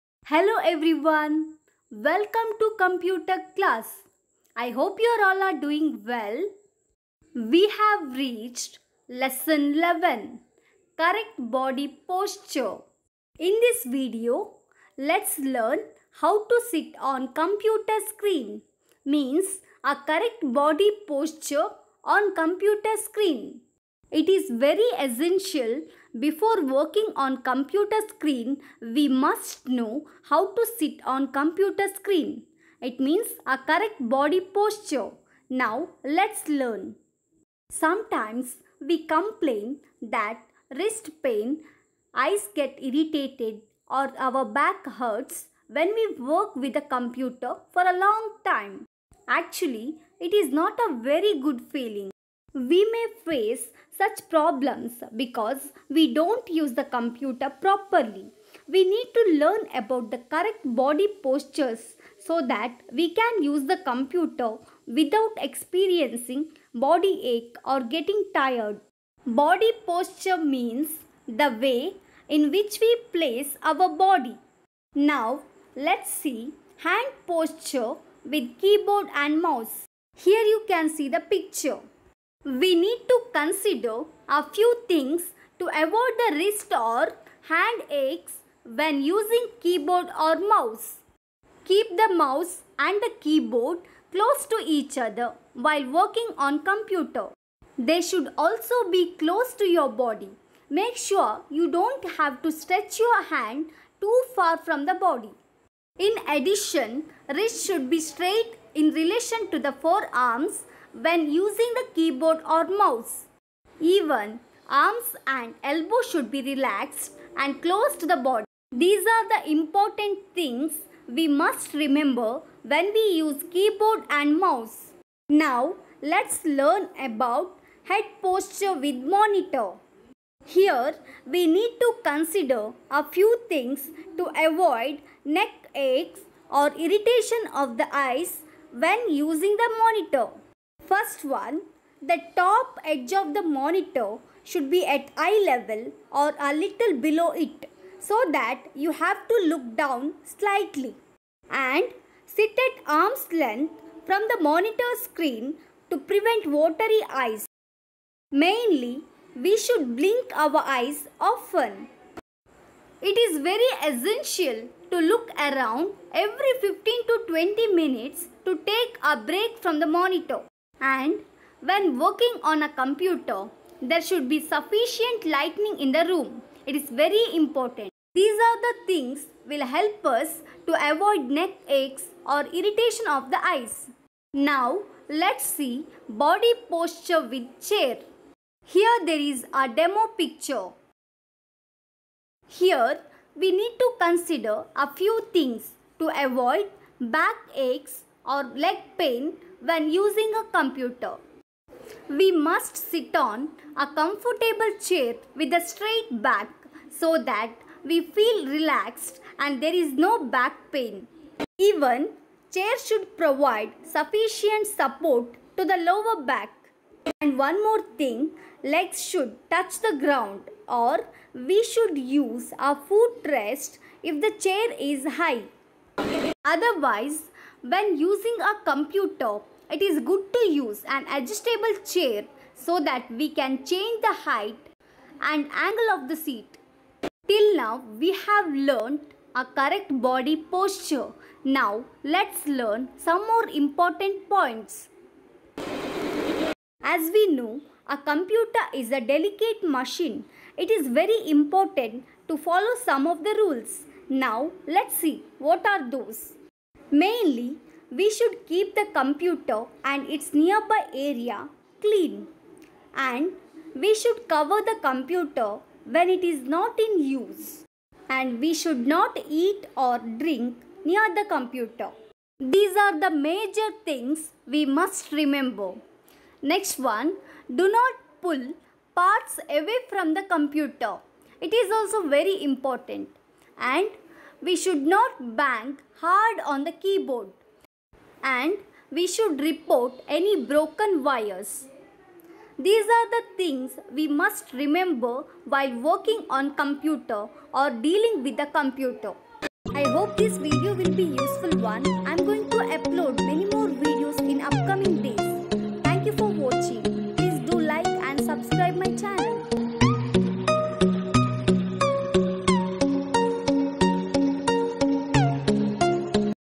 Lesson 11: Correct Body Posture
In this video , You will get information  about correct body POSTURE on COMPUTER Screen